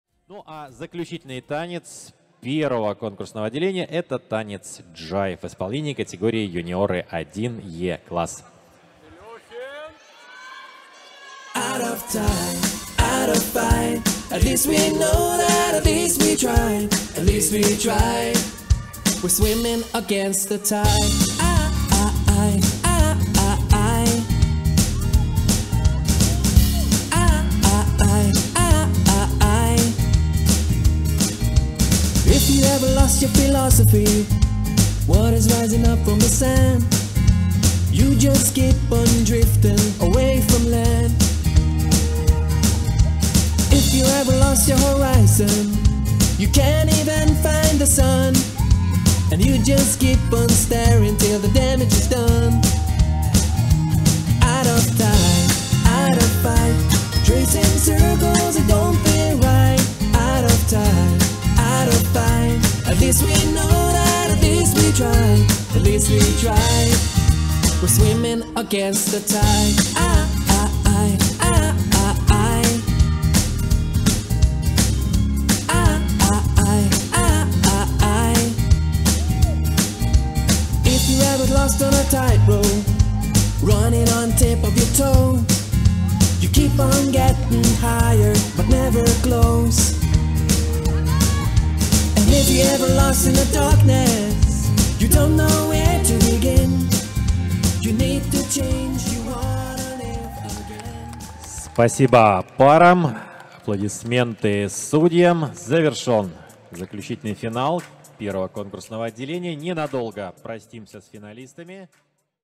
☂Юниоры 1 (до 14 лет)(Е) Финал Латина(3) танец Jive (J) CAPITAL CUP MINSK–2020
1 отделение 10:00

Финал

пара №59 Милехин Арсений и Перова Анастасия - 1 место

пара №39 Зуев Владислав и Крупич Диана - 2 место

пара №20 Клещев Никита и Рудая Юлия - 3 место

пара №21 Леонов Яромир и Шах Ангелина - 4 место

пара №37 Харитоненко Михаил и Гончарук Варвара - 5 место
(Club: Энигма, г. Фаниполь, Беларусь)
Тренер(ы): Толстик Александр и Толстик Кристина

пара №24 Малышев Виктор и Усович Анна - 6 место

пара №29 Ширко Дан и Надененко Мария - 7 место